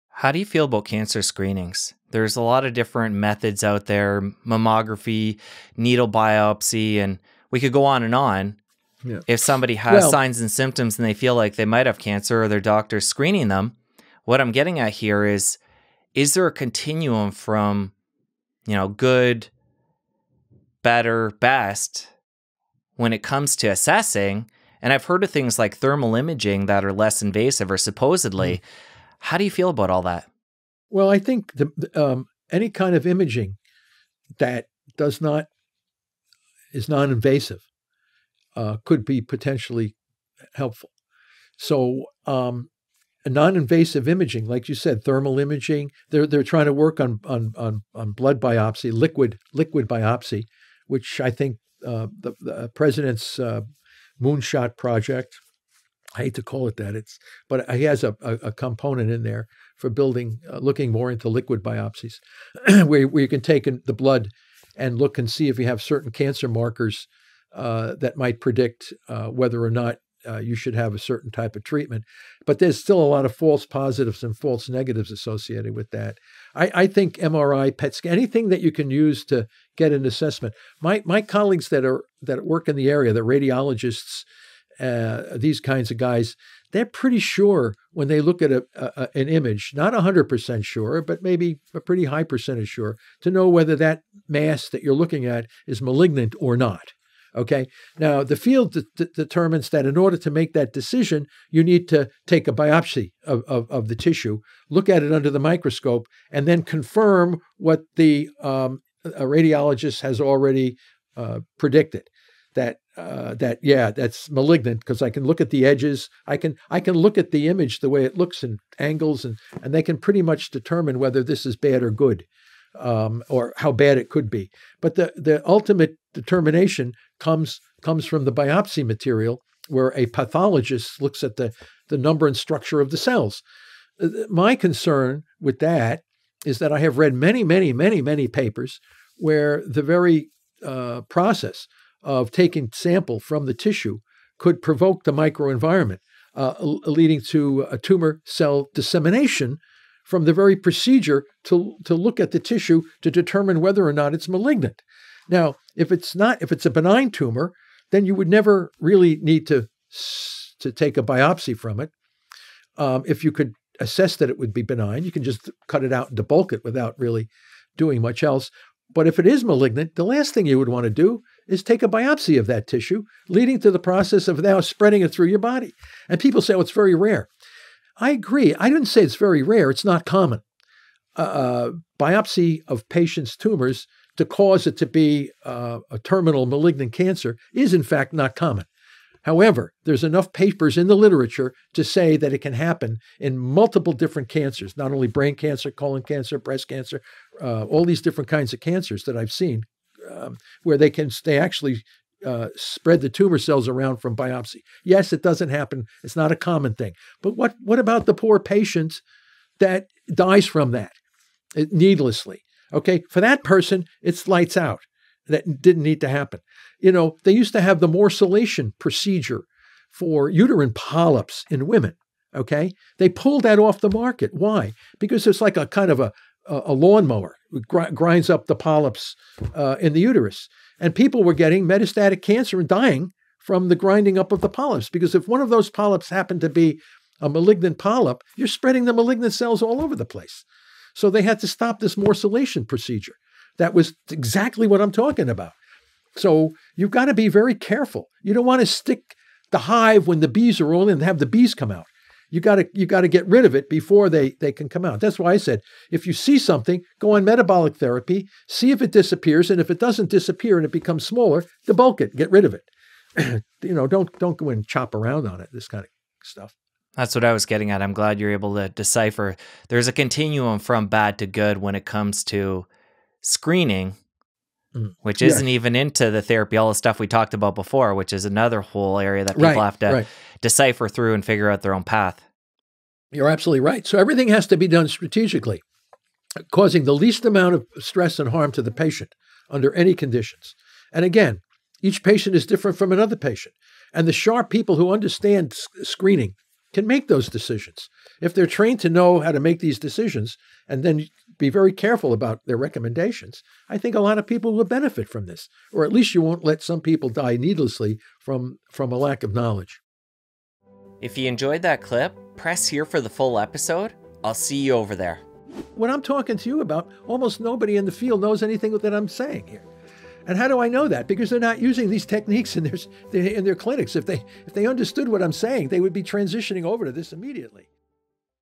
The TRUTH About Screening for Cancer | Dr. Thomas Seyfried
Thomas N. Seyfried is Professor of Biology at Boston College, and received his Ph.D. in Genetics and Biochemistry.

He has over 200 peer-reviewed publications and is author of the book, Cancer as a Metabolic Disease: On the Origin, Management, and Prevention of Cancer.

If you or someone you know has cancer, you’ll definitely want to tune in and learn about this alternative way of treating the disease.